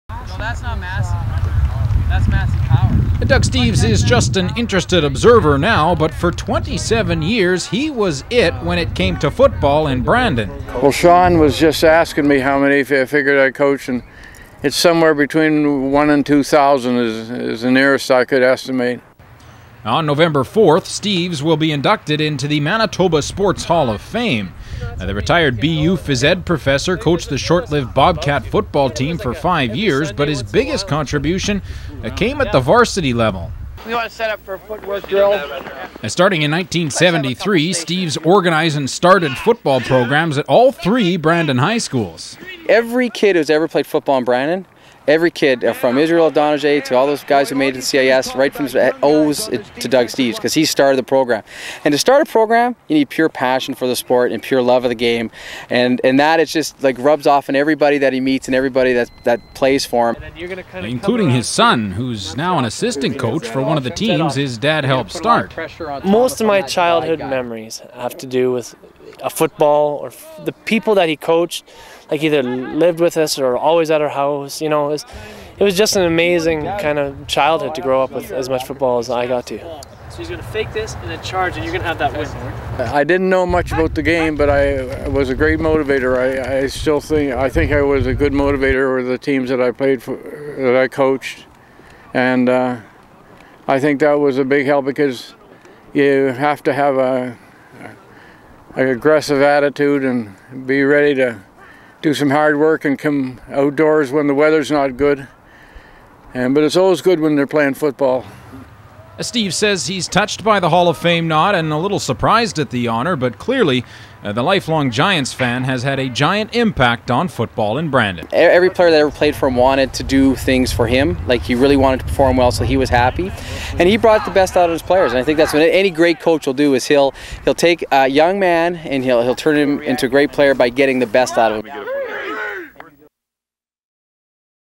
Bobcat Flashback: Doug Steeves enters Manitoba Sports Hall of Fame
Former Athletic Director Doug Steeves reflected on establishing high school football programs in Brandon and entering the Manitoba Sports Hall of Fame.